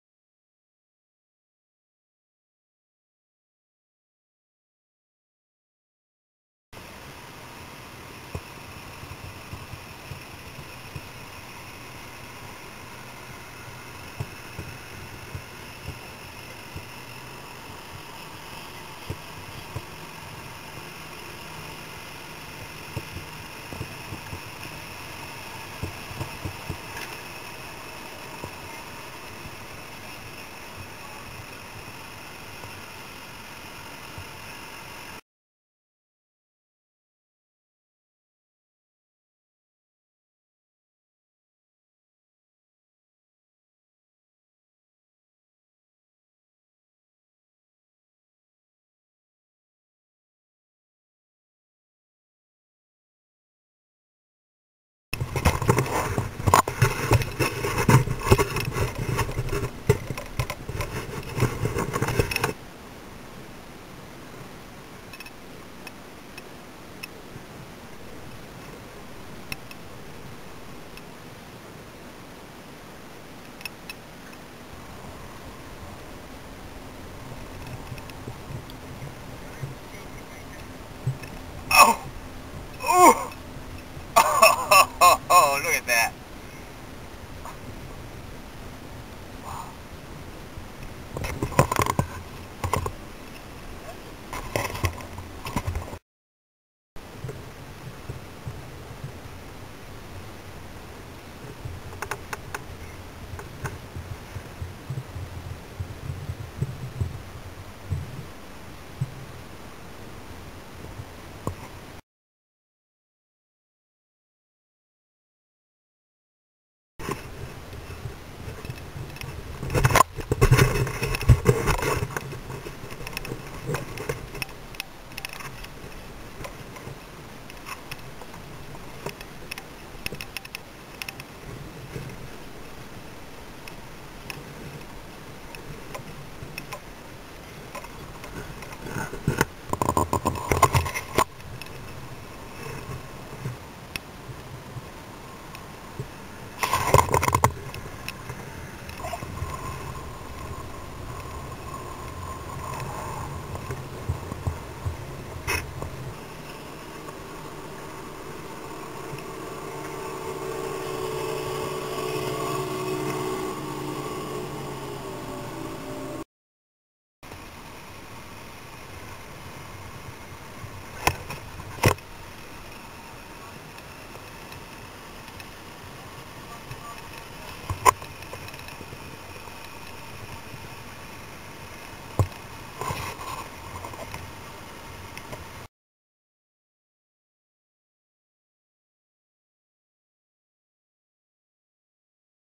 Kayak Fishing Sailing Lake Anna, VA (South of 208 Bridge) - Mar 20, 2010
Fished Large Mouth Bass and tested a new prototype sail with a boom at Lake Anna (south of 208 Bridge), VA on Mar 20, 2010.

This was my last fresh water fishing this year before I hit Chesapeake Bay. Saw a LM at 6 7/8 lb by young kids.

The Boom improved the sailing speed by 20%. But I need to come up with the new procedure for self rescue in the rough water when the kayak capsizes.